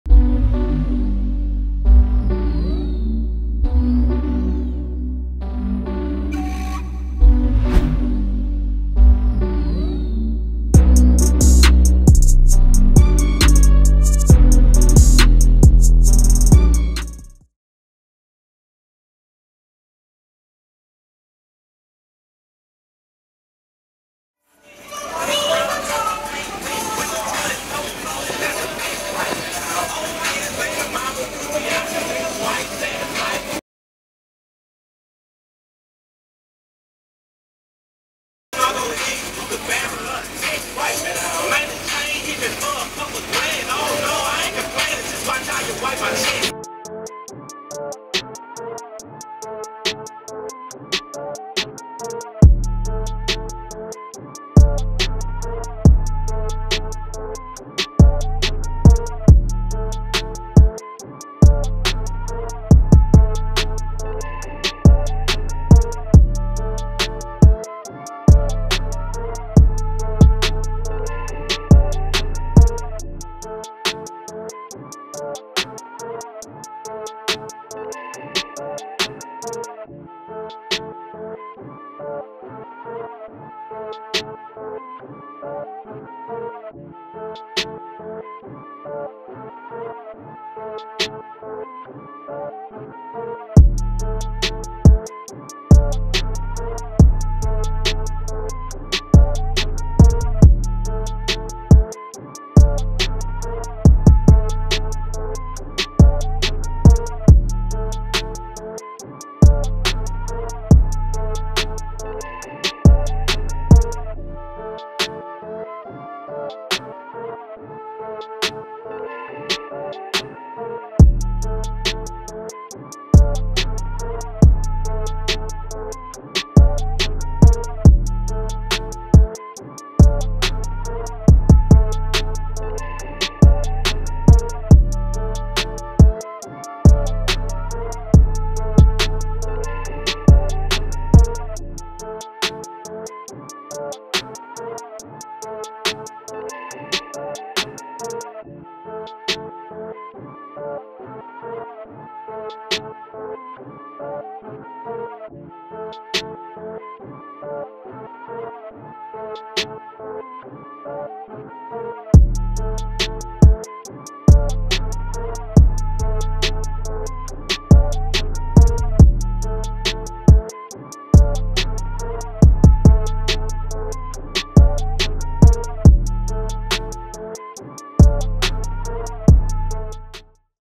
WhipAddict: Lucky's Legendary 71' Impala Convertible, 632 Big Block, Kandy Fade, Big ZL1 Brakes
@Lucky71_vertkiller brought his Legendary 71' Donk up to Macon from Miami to show the city want the Donk Captial brings to the table. This Donk has been around over 11 years, including big Chevrolet Brakes from 2009, but refreshed the Kandy Fade Gold paint job, football color interior and top, with the custom trunk, big block 632 motor with NOS all over it, sittin' on color matched 26s! I'm probably missing some things but you get the picture.

@whip_addict Instagram

For Car Show Calendar and Gear